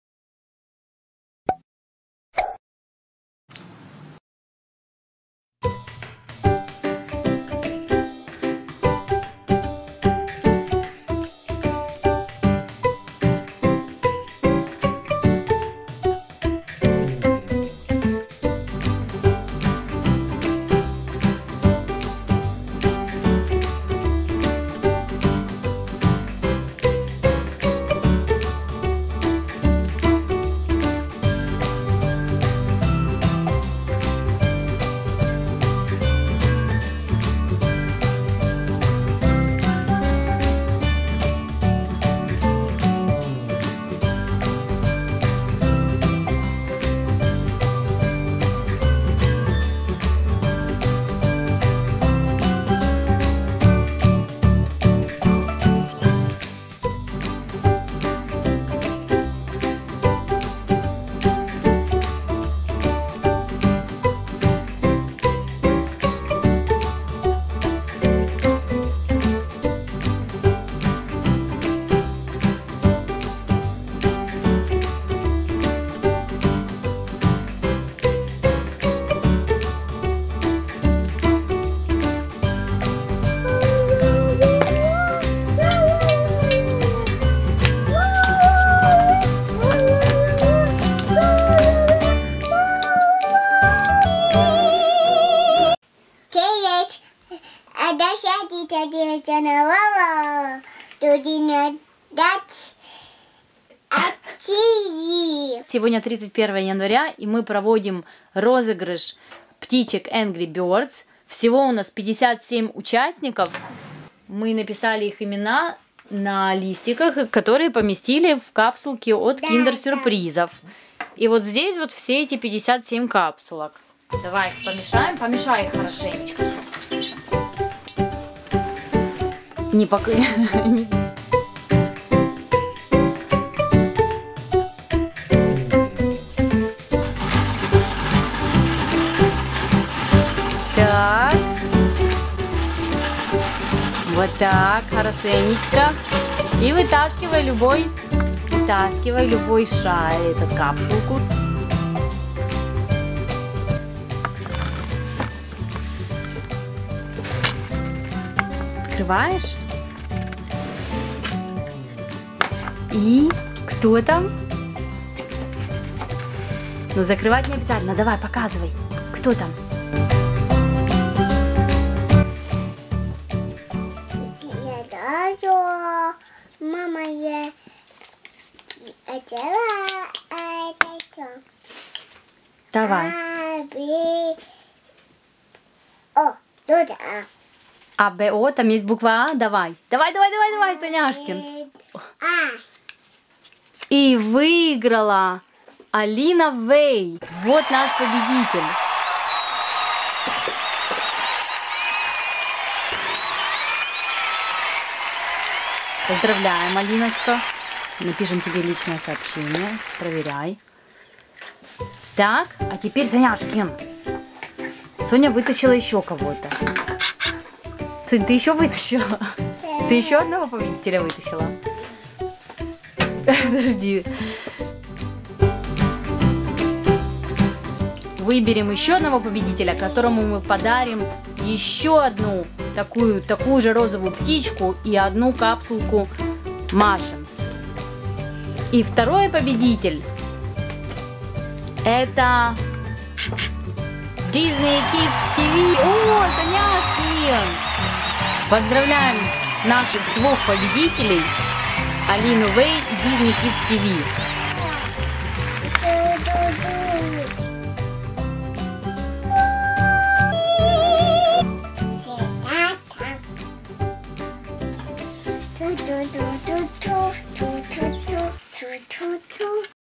Розыгрыш ЭНГРИ БЕРДС Машемс | Объявляем победителя - GIVEAWAY WINNERS Angry Birds Mashems
Сегодня 31 января, и мы провели розыгрыш Энгри Бердс Машемс (Angry Birds Mashems), который анонсировали
Поздравляем победителей!
Мы с вами свяжемся для получения адреса отправки.
Спасибо всем за участие!

International Drawing of Mashems Angry Birds.